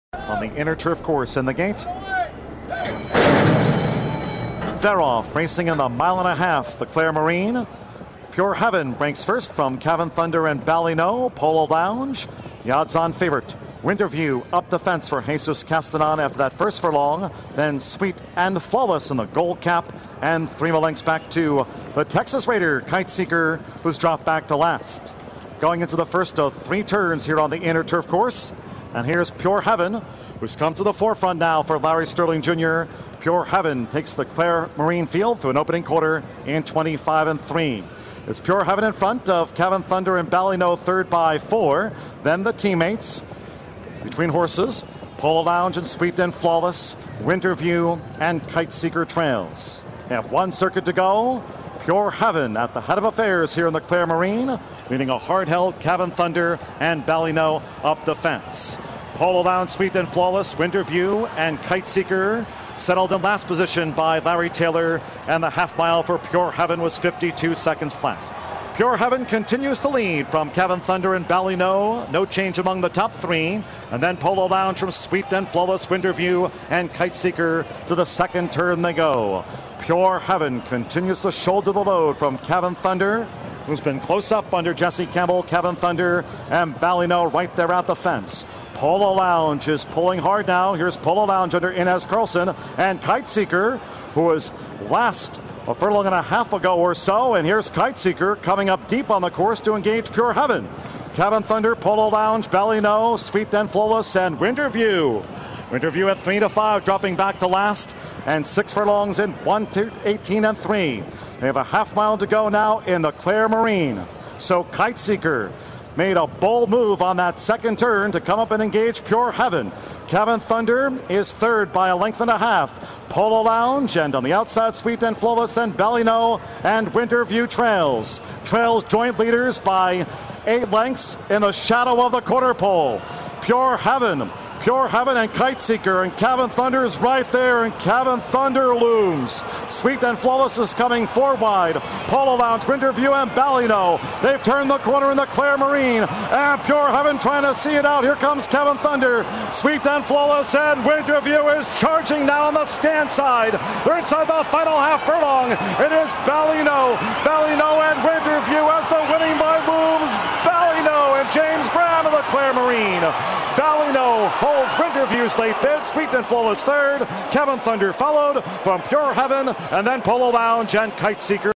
ARLINGTON PARK, 2009-09-19, Race 7
Entries: Thoroughbred Race, ARLINGTON PARK, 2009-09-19, Race 7:
      (Pgm-Runner-Jockey-Finish)
       1A-Polo Lounge (KY), Karlsson, I, finished 6;
       1X-Sweet and Flawless (KY), Alvarado, J, finished 3;
       2-Ballynoe (KY), Graham, J, finished 1;
       3-Winter View (KY), Castanon, J, finished 2;
       4-Cavan Thunder (KY), Campbell, J, finished 4;
       5-Pure Heaven (KY), Sterling, Jr., L, finished 5;
       6-Kite Seeker (TX), Taylor, L, finished 7;